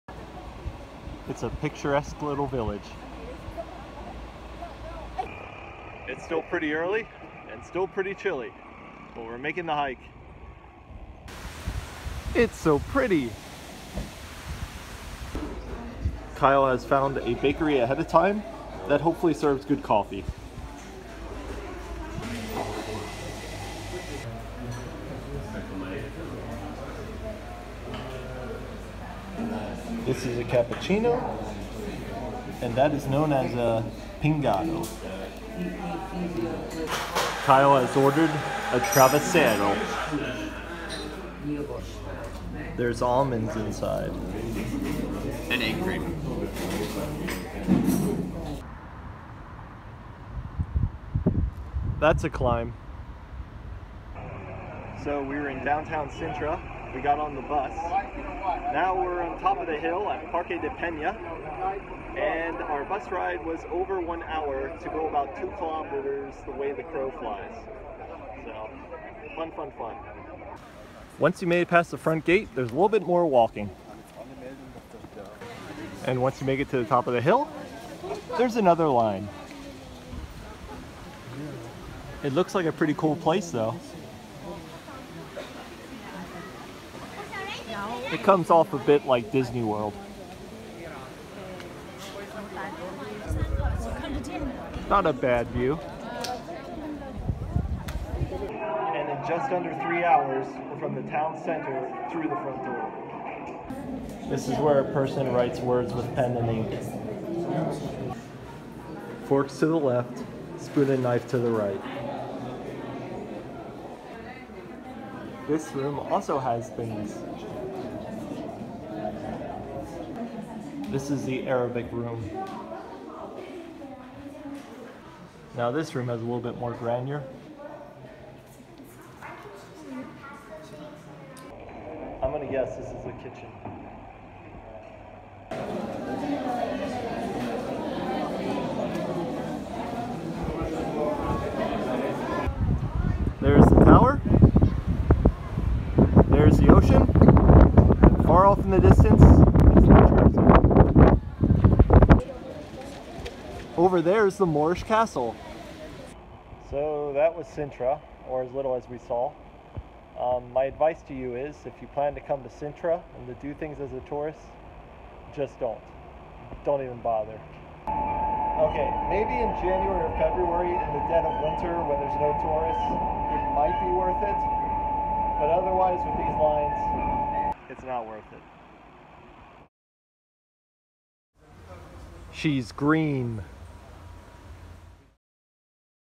A TRAVESSEIRO & MORE IN SINTRA, PORTUGAL
My cousin and I spend the day in Sintra in order to see downtown and Pena Palace. Long lines await you whatever you choose to see here. But it does not seem to deter many people.